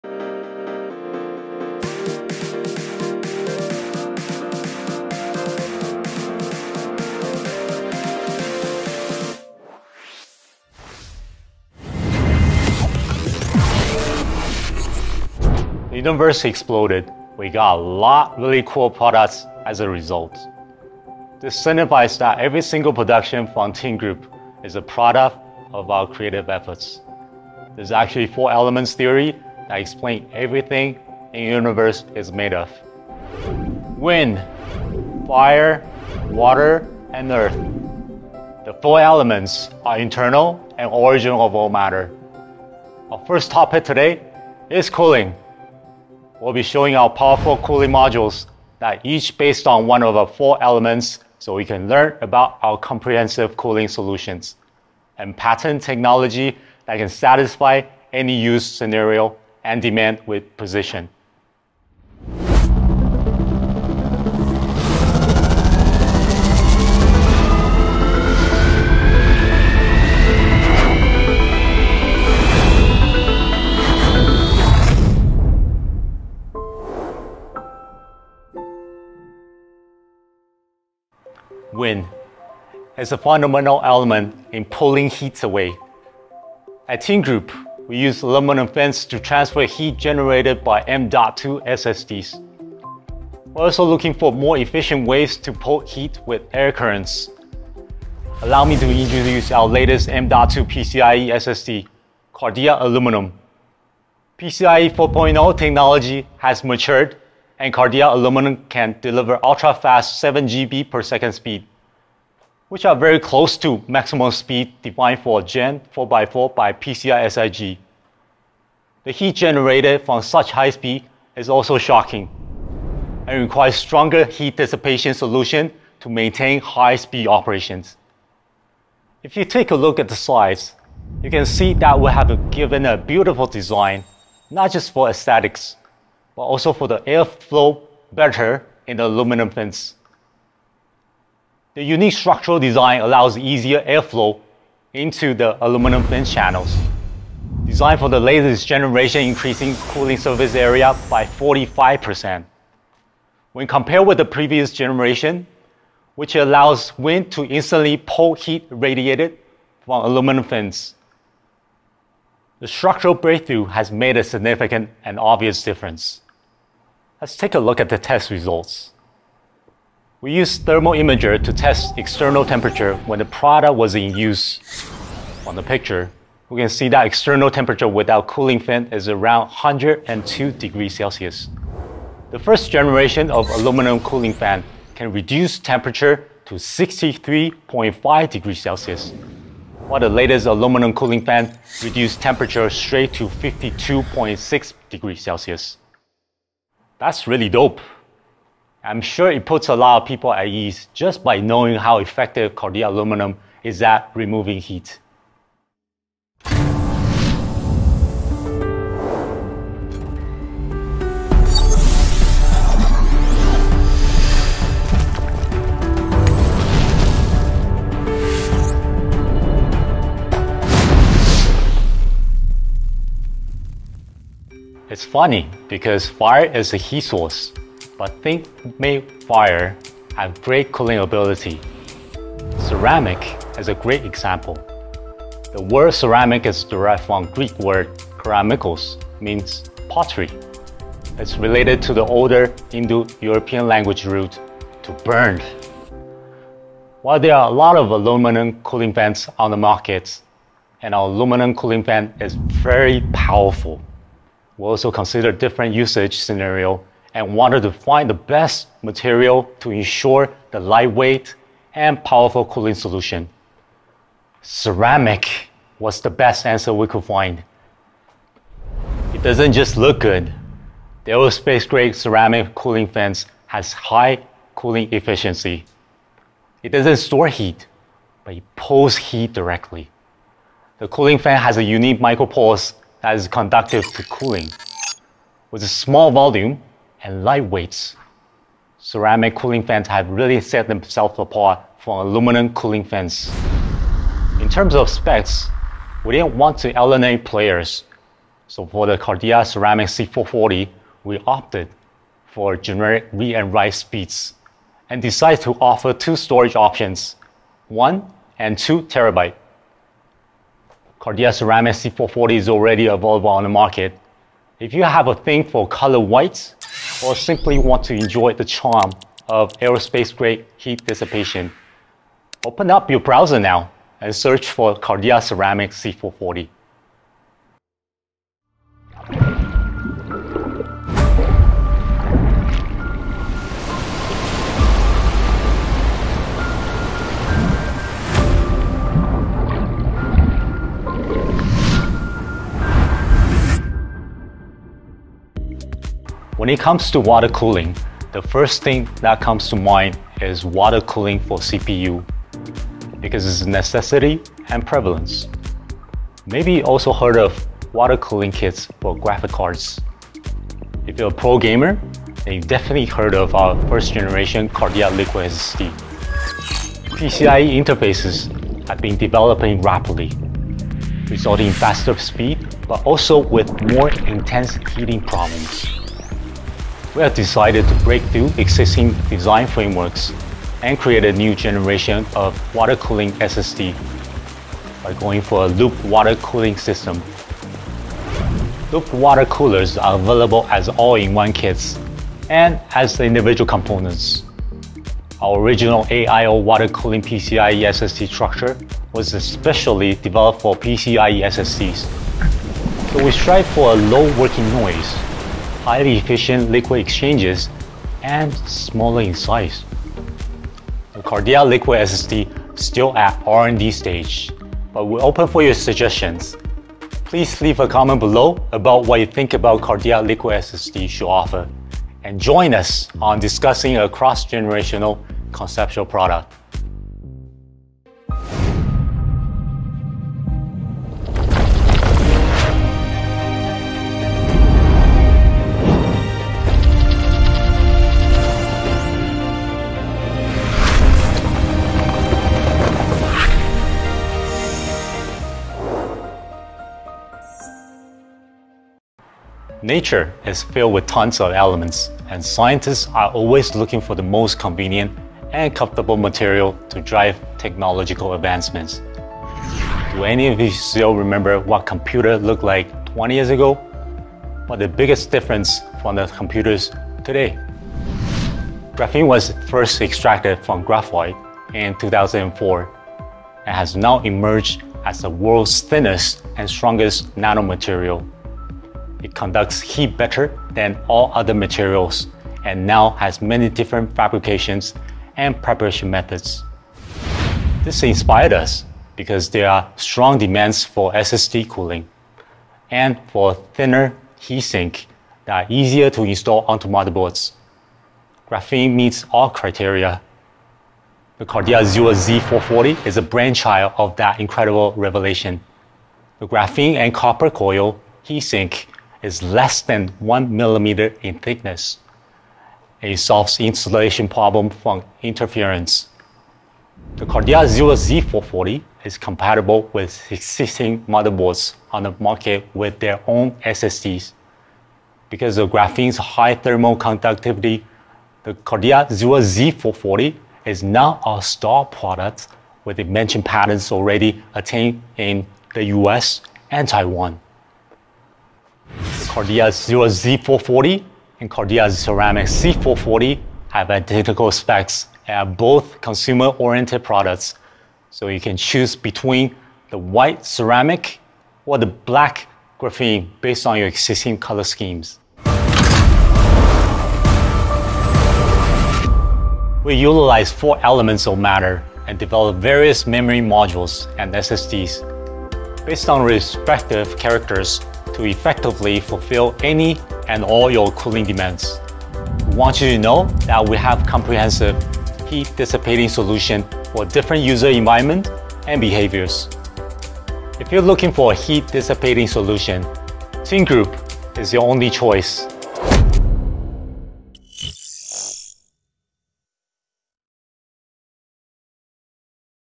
TEAMGROUP Diverse Heat Dissipation M.2 SSD & AIO CPU Cooler
CARDEA A440 Pro PCIe uses improved aluminum fin thermal conductivity for fluid ventilation and effective cooling performance.
CARDEA C440 PCIe’s aerospace ceramic heat sink with tiny pores, can prevent from high thermal design power rating.
SIREN AIO ARGB AIO Cooler uses looped liquid cooling system to provide higher cooling performance.
CARDEA Z440 PCIe’s graphene heat sink, measured less than 1mm in thickness, effectively eliminates interference during installation.
CARDEA A440 PSS PCIe designed for PS5 storage expansion, is the first to introduce white graphene heat sink.